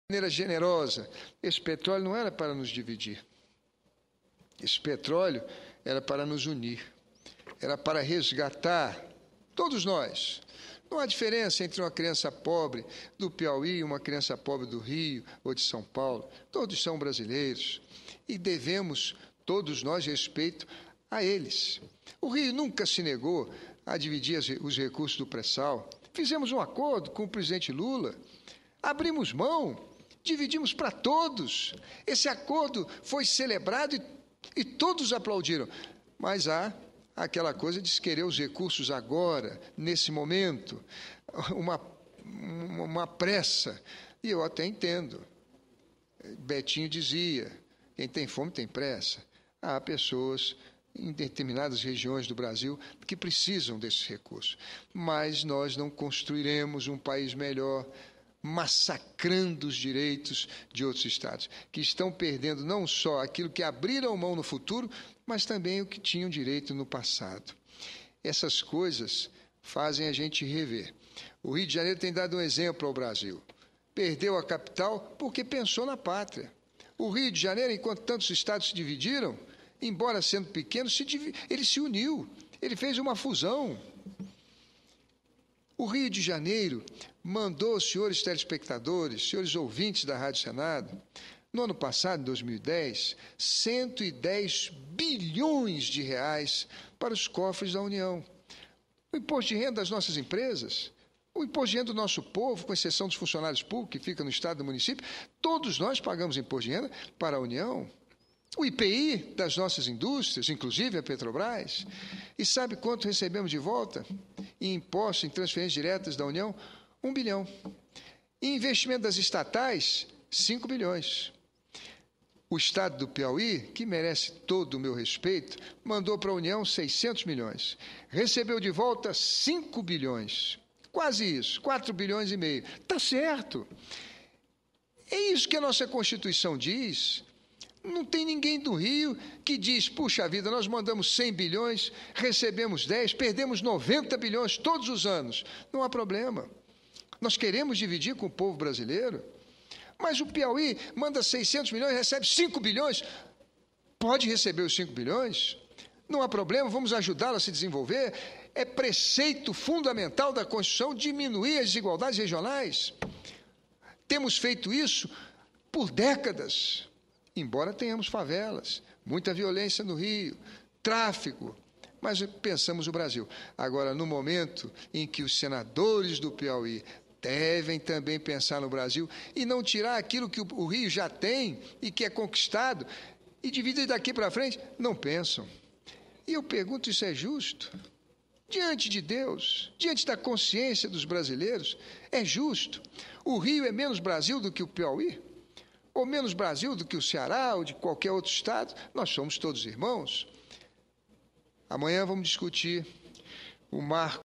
"Esse petróleo não era para nos dividir, era para nos unir", adverte sen. Marcelo Crivella
18/10/2011 - Plenário - "Esse petróleo não era para nos dividir, era para nos unir", adverte sen. Marcelo Crivella